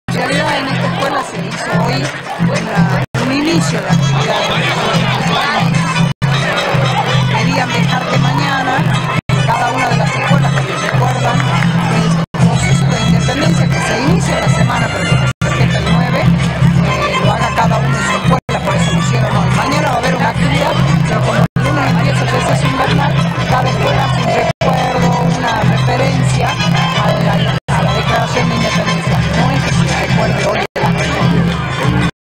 Desfile 9 de julio
Ministra de Educación en el desfile organizado por la escuela Nº 4029 “Dr. Nicolás Avellaneda” Creadora de la Bandera de Salta